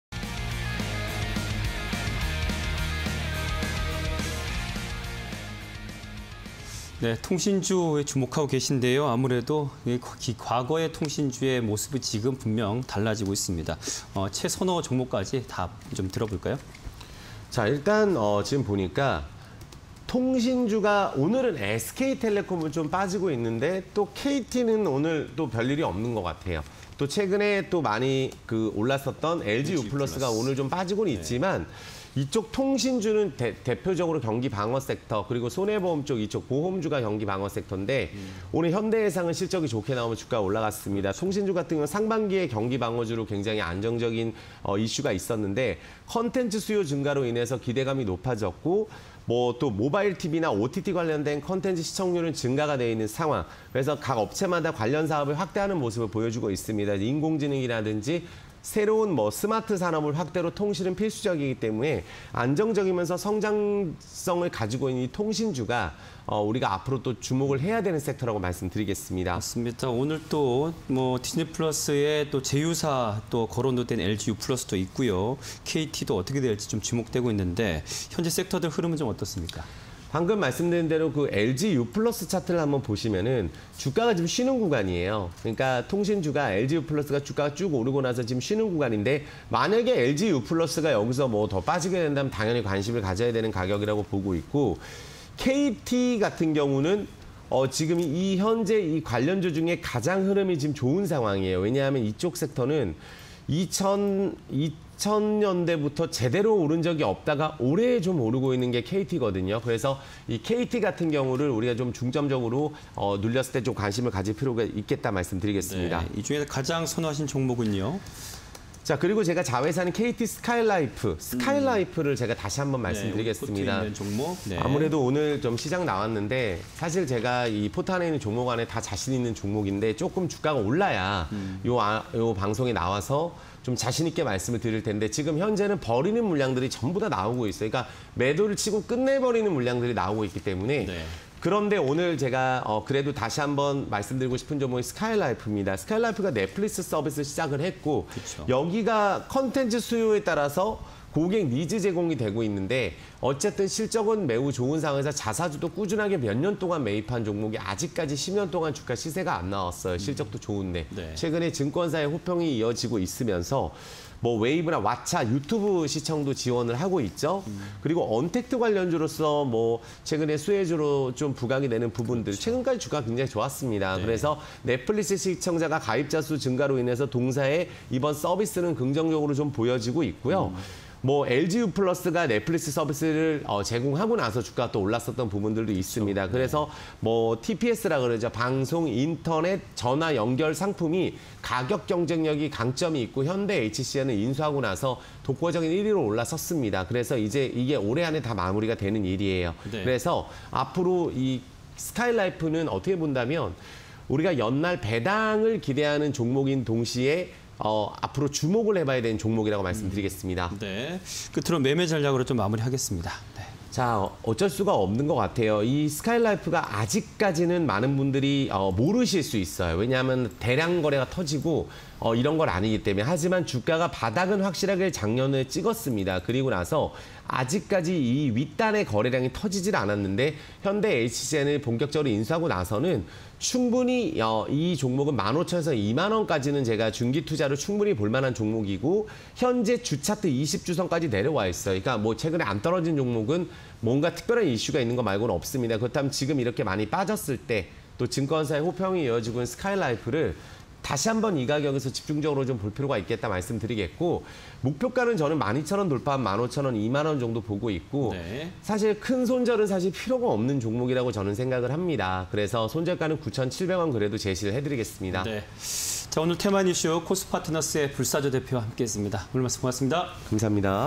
스카이라이프(053210)... 통신주_테마IN이슈 (20210817)
- 대표적 경기 방어주
- 상반기, 경기방어주 '매력'… 안정적 우상향
- 지난해 대비 실적 기대감 증대
- 콘텐츠 수요 확대… 기대감 확대
- 모바일ㆍTV 등 OTT 통한 콘텐츠 시청율 증가
- 새로운 스마트 산업 확대… 통신 필수불가결
- 안정적ㆍ성장동력 갖춘 사업성 보유

스카이라이프(053210)
- 넷플릭스 서비스 시작
- 새로운 콘텐츠 수요 증가… 고객에게 '니즈' 제공
- 자체 OTT 플랫폼 통해 '웨이브, 왓챠, 유튜브' 등 지원
- 과거 LG유플러스, '넷플릭스' 서비스 후 매출 증가
- 자회사 '스카이TV', 자체 PP 제작 역량 보유
- TPS 부문 가격 경쟁력
- 합리적인 소비에 대한 선호도 증가
- 'SKY TPS' 가입자 순증… 가격 경쟁력